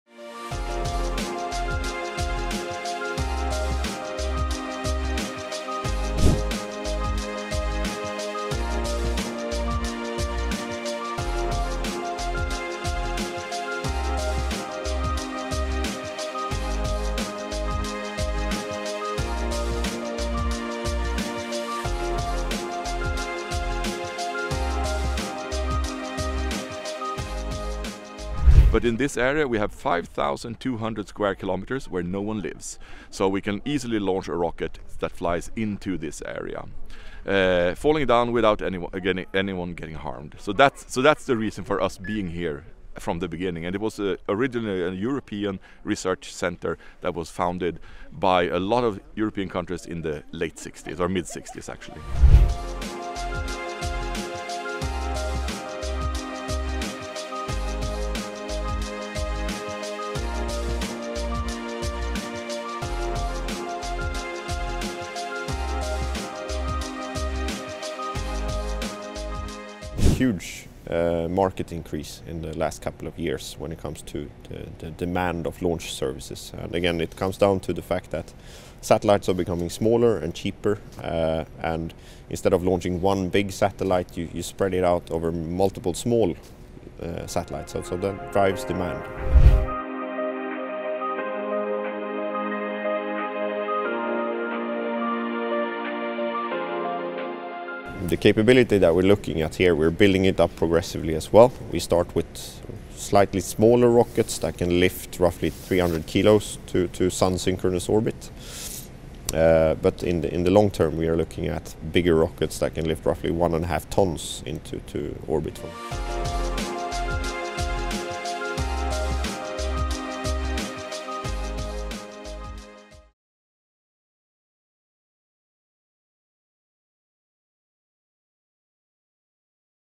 Des fusées dans l’Arctique : la Suède se prépare au lancement de satellites
A 200 kilomètres au-dessus du cercle polaire arctique, le centre spatial d’Esrange, dans le nord de la Suède, espère lancer ses premiers satellites dès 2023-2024. En effectuant des tirs sur le sol européen, les initiateurs du projet misent sur la proximité avec les clients du continent. La station est en discussion avec plusieurs concepteurs de fusées. (Avec AFP)